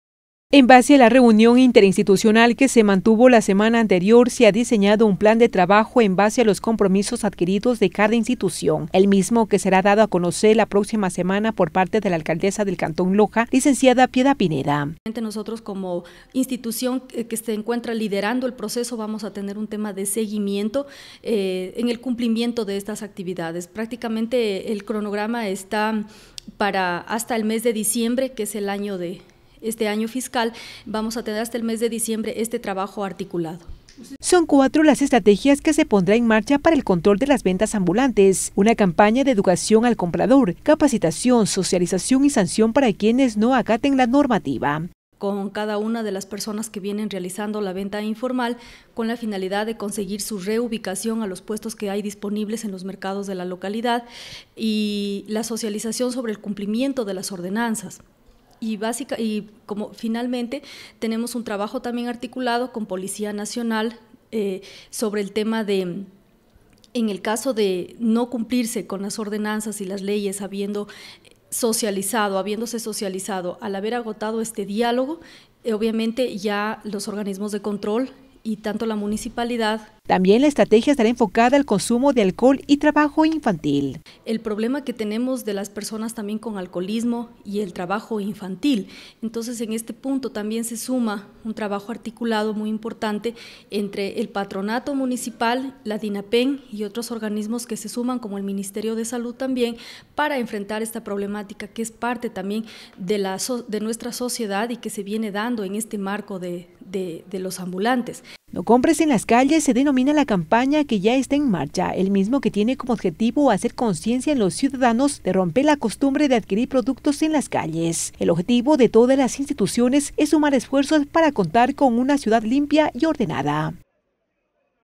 Campaña no compres en la calle
En base a la reunión interinstitucional que se mantuvo la semana anterior, se ha diseñado un plan de trabajo en base a los compromisos adquiridos de cada institución, son 4 las estrategias que se pondrá en marcha para el control de las ventas informales.